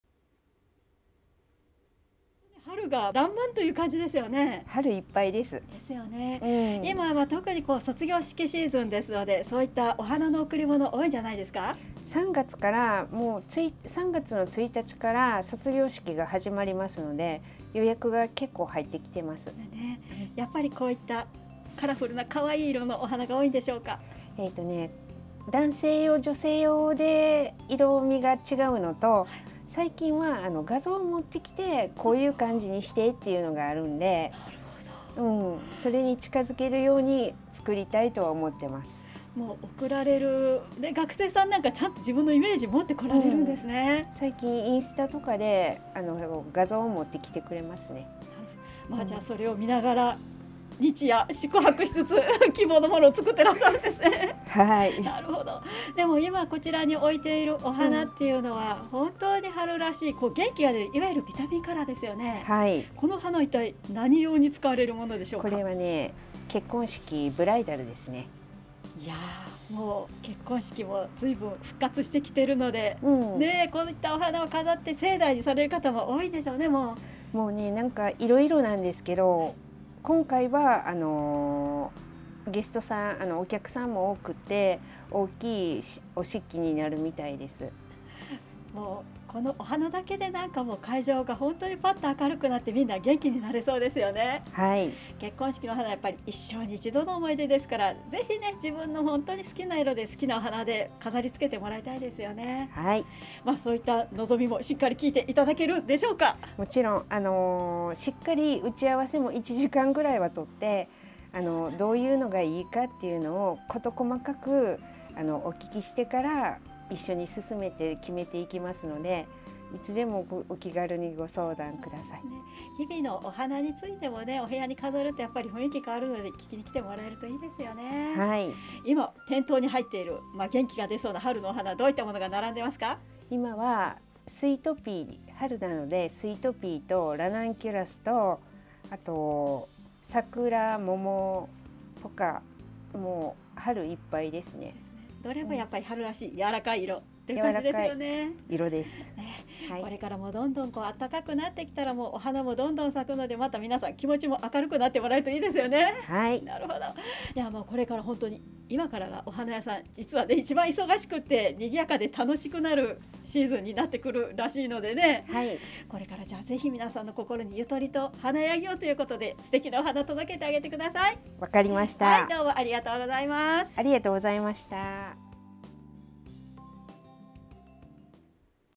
≪お店情報≫フラワーガーデン花工房
今回のお店情報は、坂出市中央町の「フラワーガーデン花工房」をご紹介します。
※映像内の金額・キャンペーン等は、取材時の内容です。